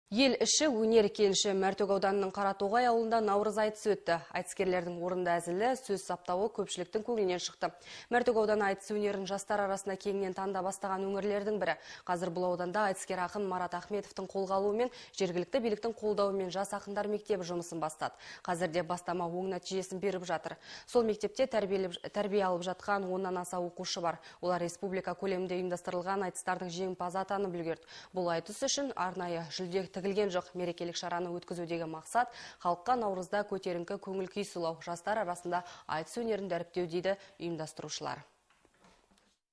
Қаратоғай ауылында наурыз-айтыс өтті
«Ел іші - өнер кеніші». Мәртөк ауданының Қаратоғай ауылында наурыз айтыс өтті. Сайт